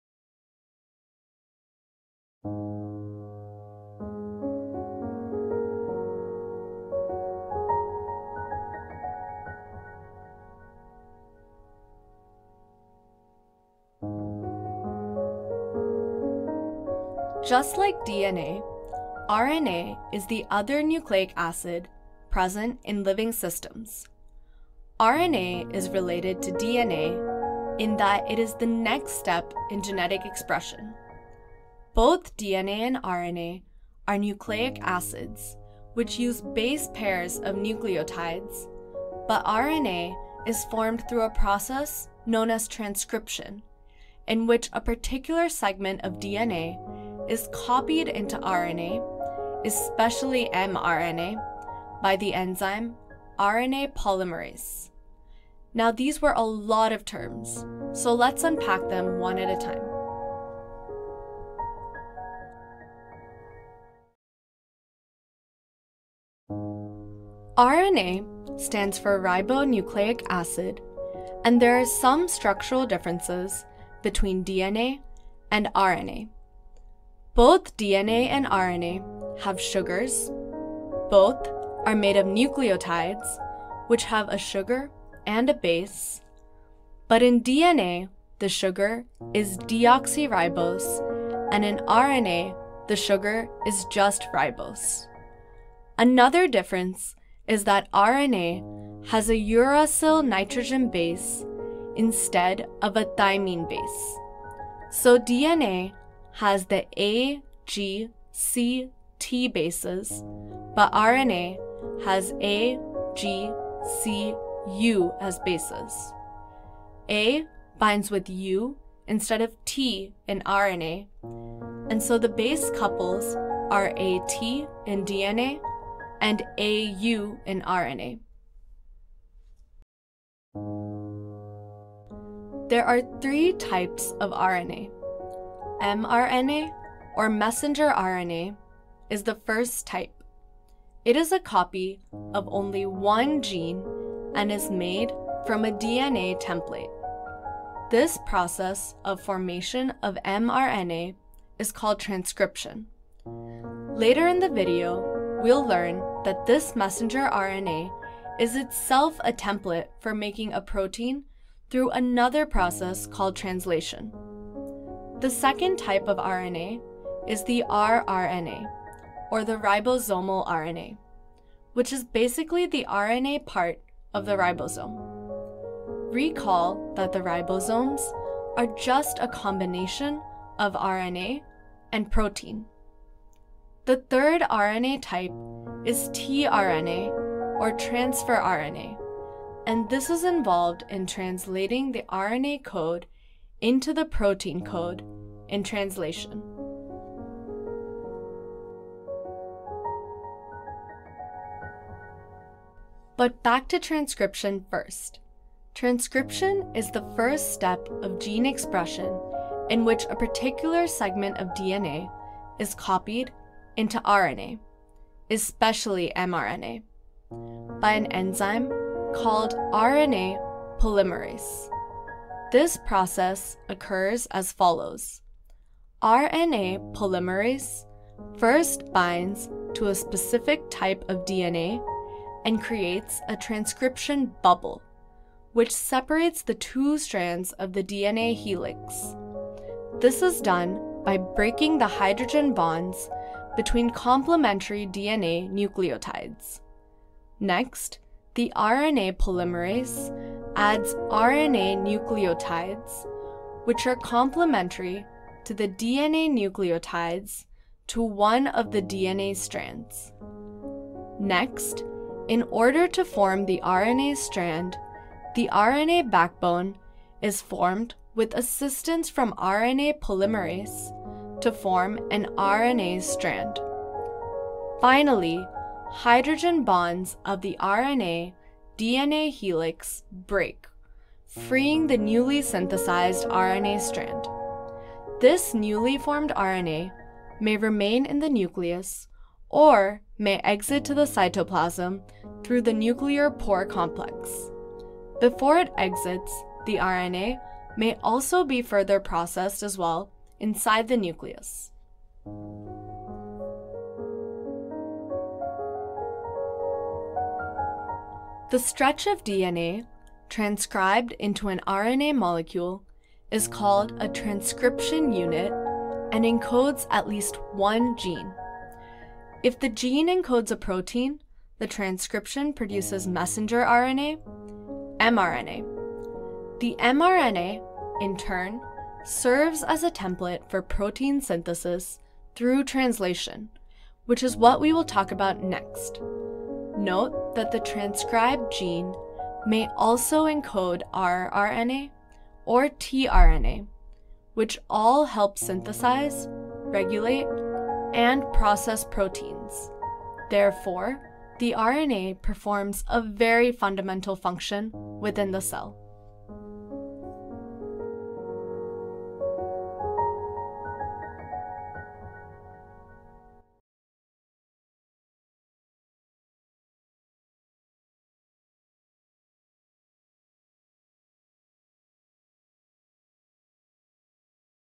Chapter 7: RNA and Transcription
This is the seventh sample video in the AP Biology VR series, created in collaboration with the School of Interactive Computing at Georgia Tech. For more such sample videos, be sure to follow our YouTube channel!